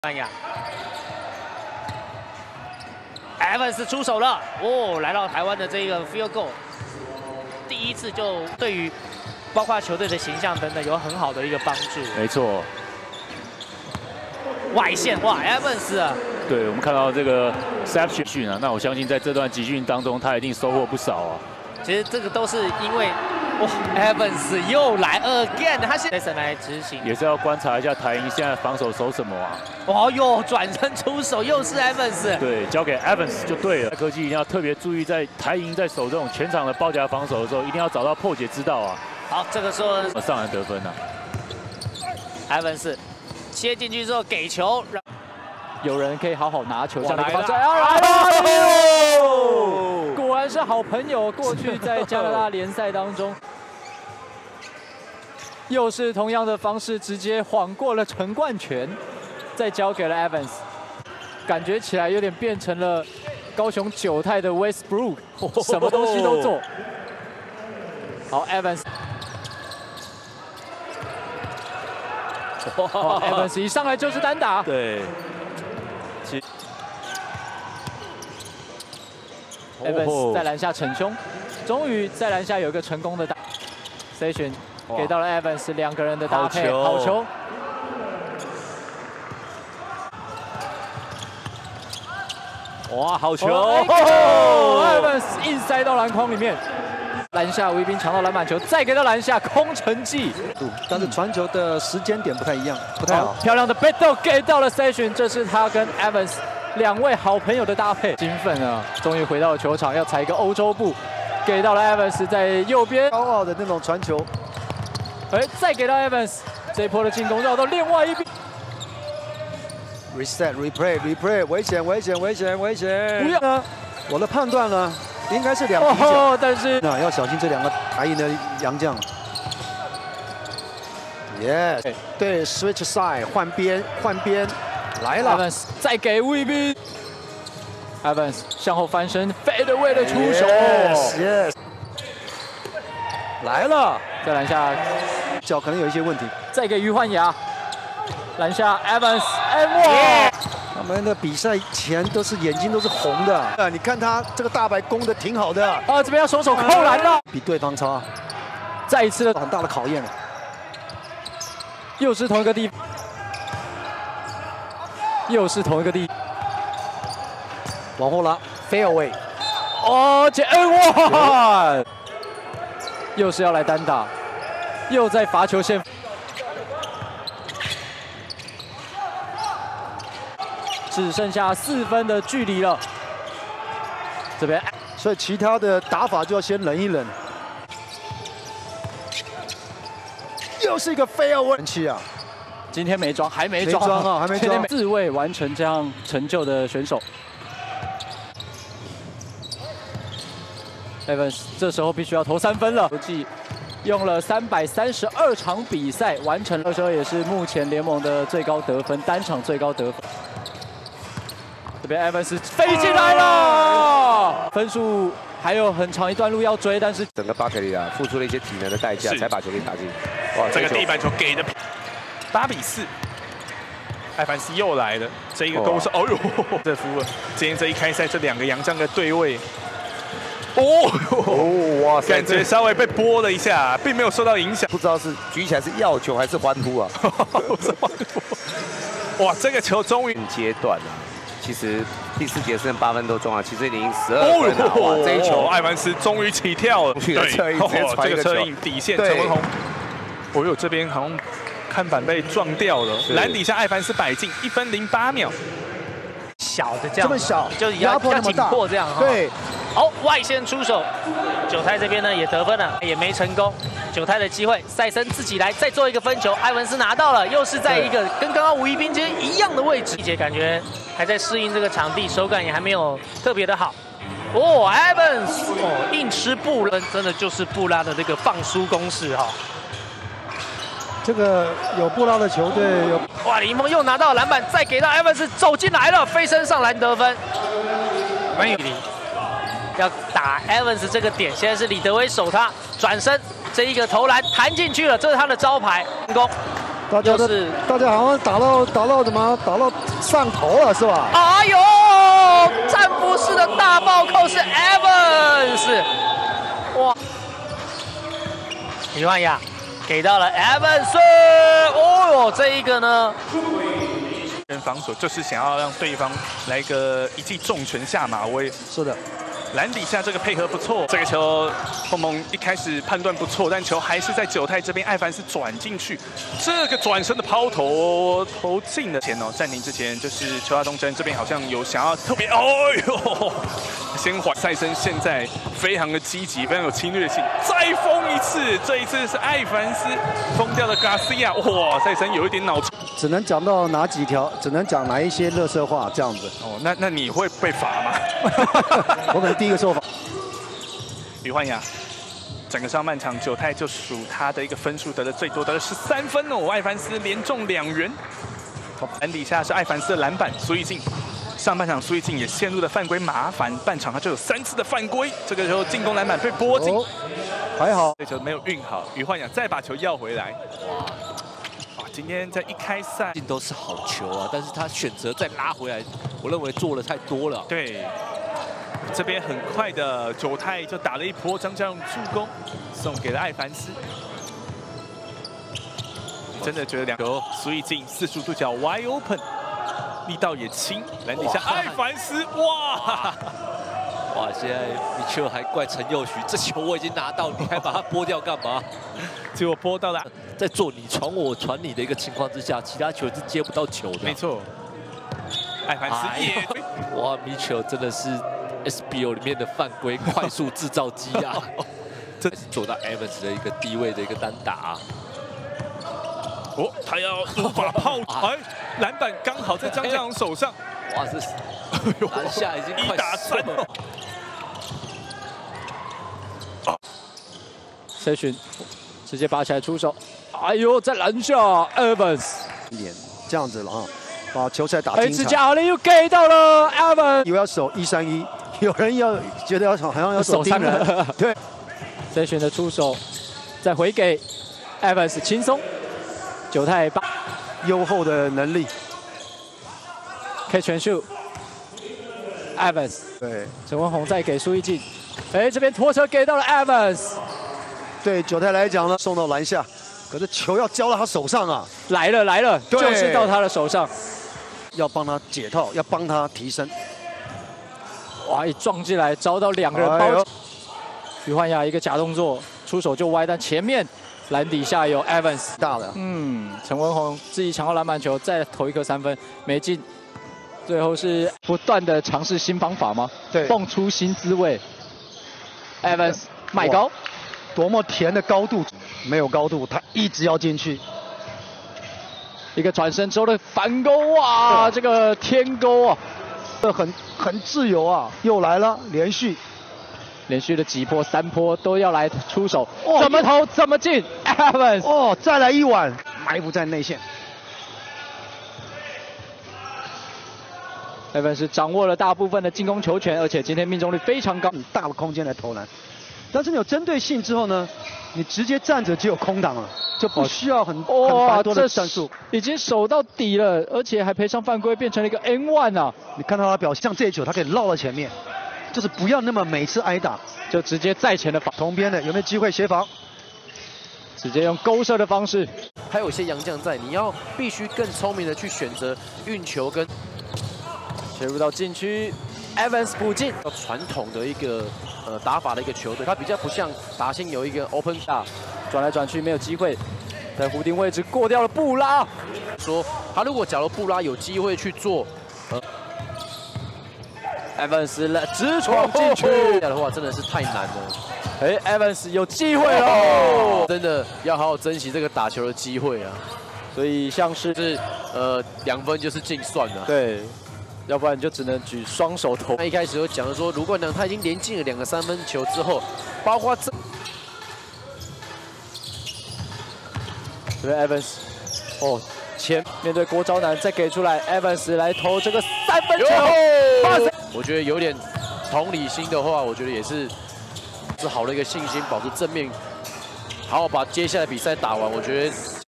2019-2020 SBL Jeou-tai Tech #45 Nick Evans 18.3PTS 8.9REB Season Highlights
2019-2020 Super Basketball League (SBL) Jeou-tai Tech #45 Nick Evans 18.3PTS 8.9REB Season Highlights